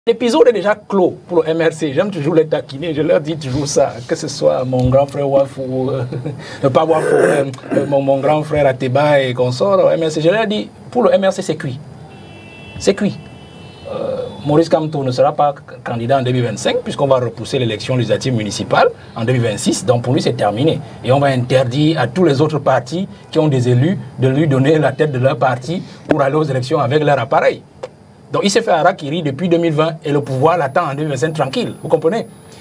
MRC : MAURICE KAMTO Ne Pourra Plus être Candidat aux Prochaines Élections
MRC : MAURICE KAMTO Qui a Refusé de Participer Aux Élections 2018
Il ne Pourra Plus être Candidat aux Prochaines Élections
  Journaliste Yaoundé, Cameroun